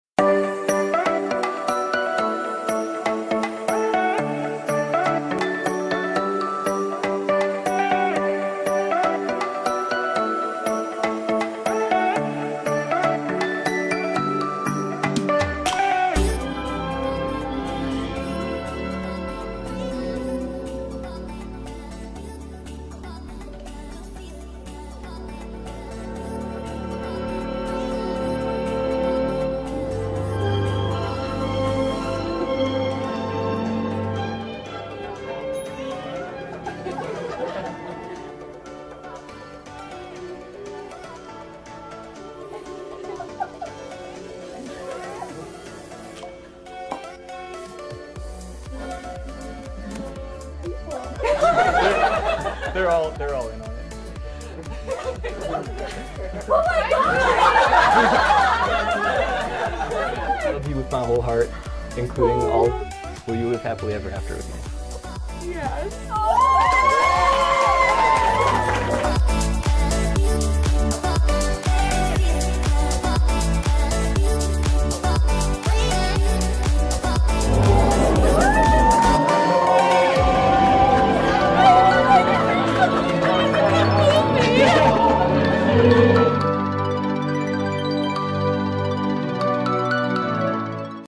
La plus incroyable demande en mariage ... 💍 👰🏼🤵🏼😍
La plus incroyable demande en mariage... qu'on ait jamais vu... et pourtant, on en a vu ! Regardez !  👰🏼💍🤵🏼😍